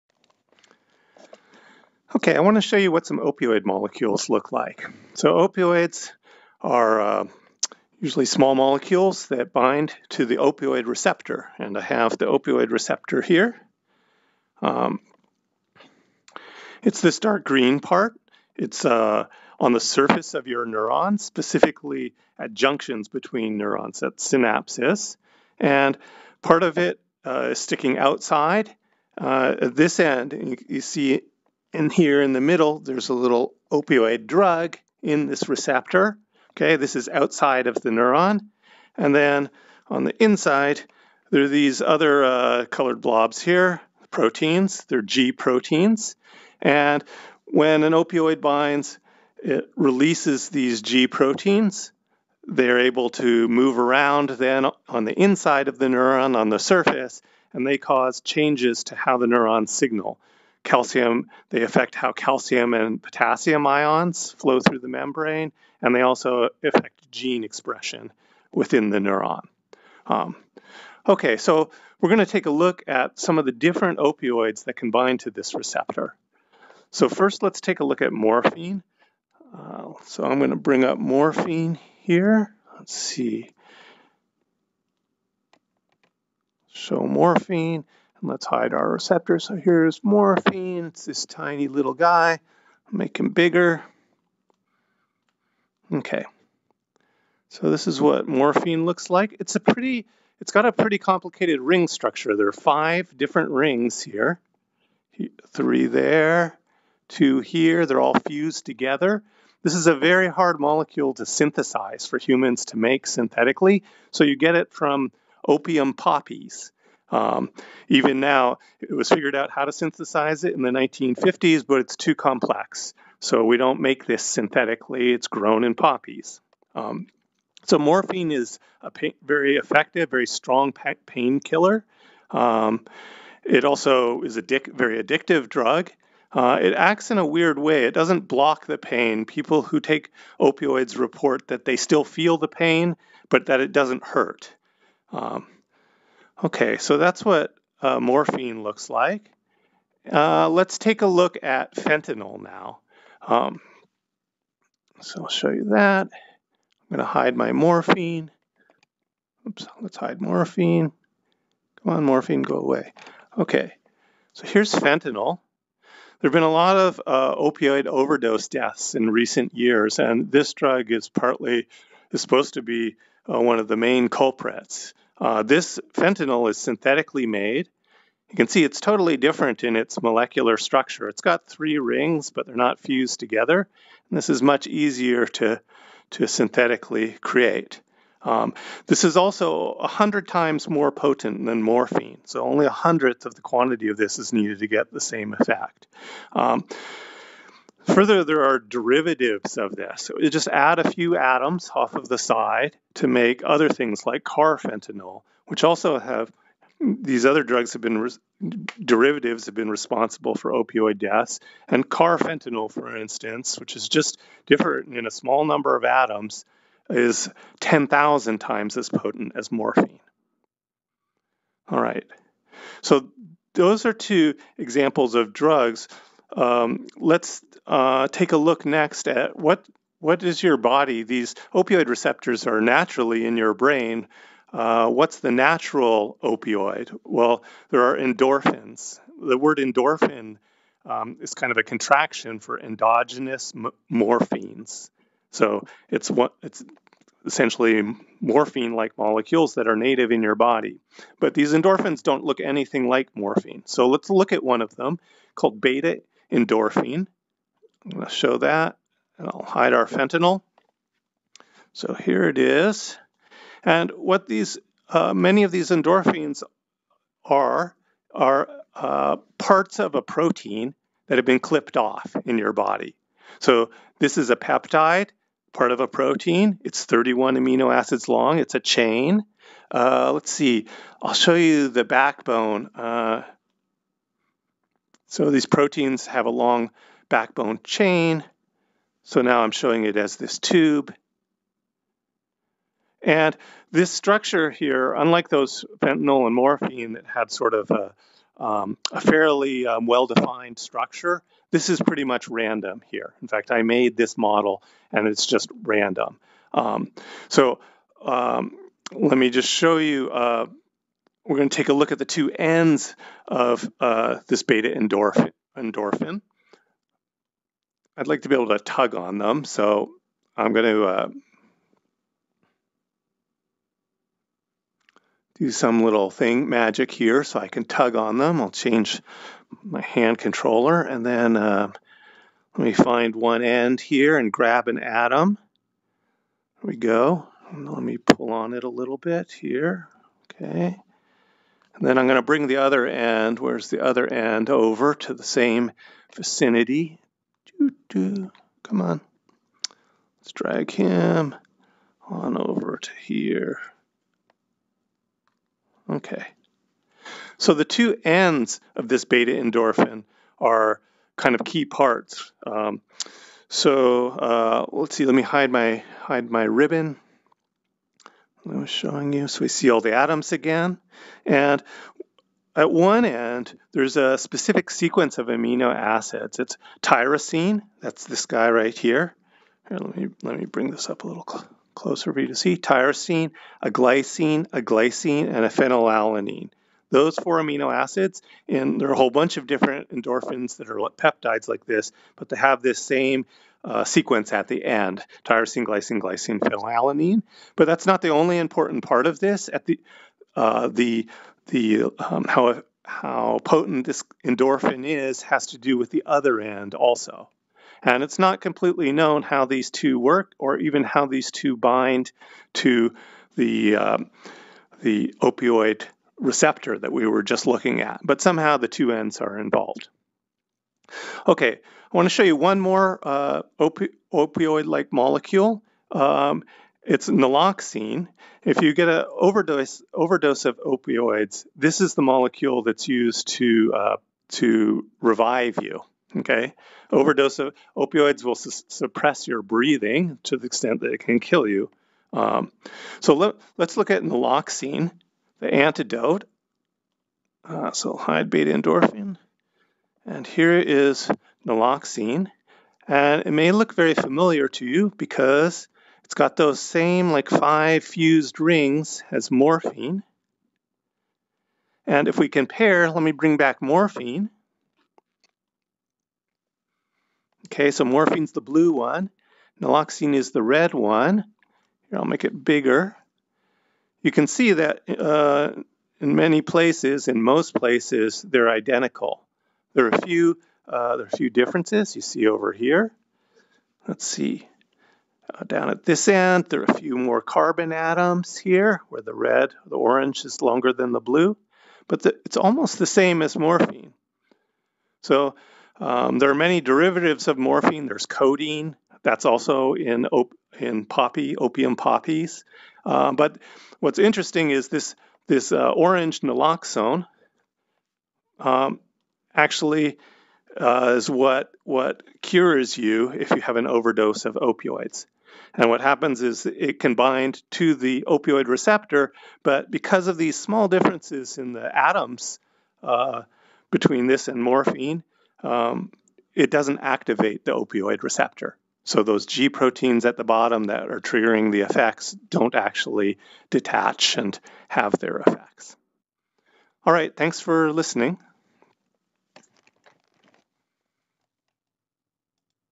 Opioid Molecules Mixed Reality
We take a look at opioid molecules morphine, fentanyl, beta endorphin and naloxone and the mu-opioid receptor using virtual reality.

This uses ChimeraX VR software.  The mixed reality video was captured using an Intel RealSense depth sensing camera and is described here